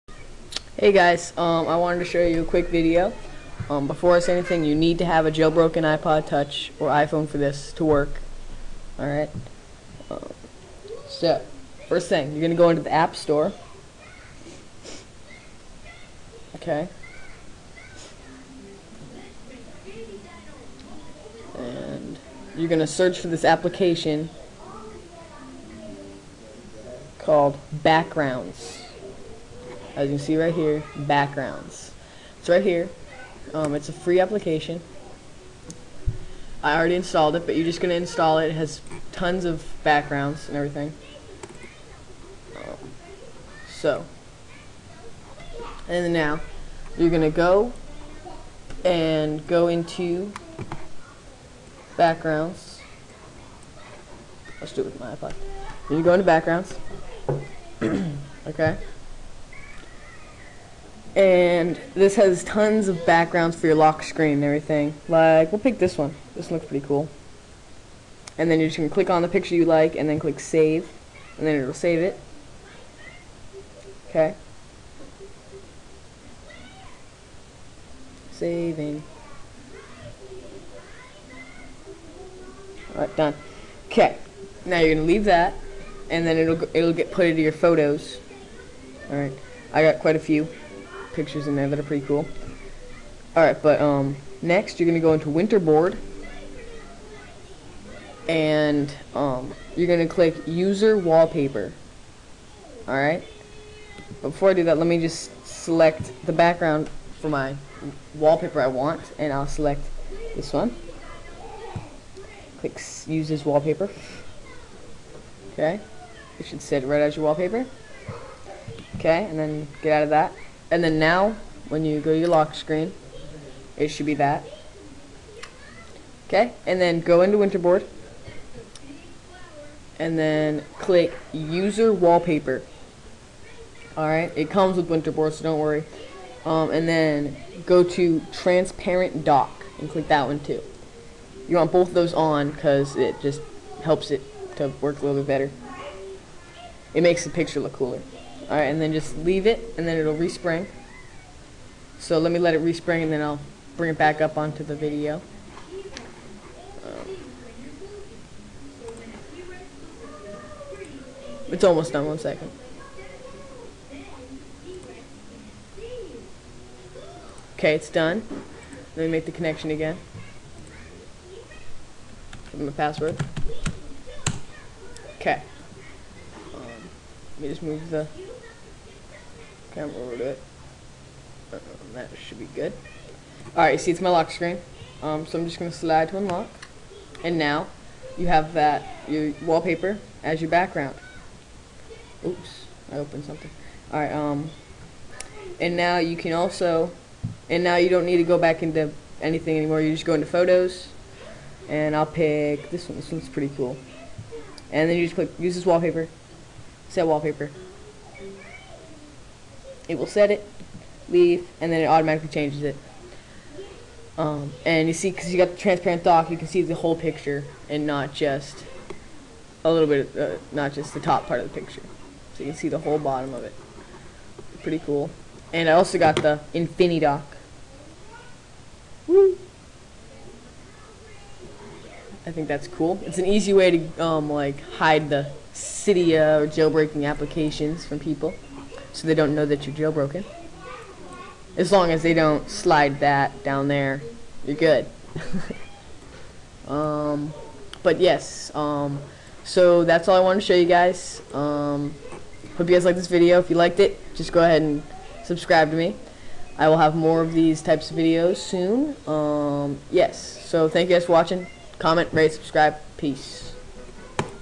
how to get cool background pics on ipod touch/iphone
this is a tutorial on how to get cool background pics on an ipod touch/iphone. you need to be jailbroken to do this!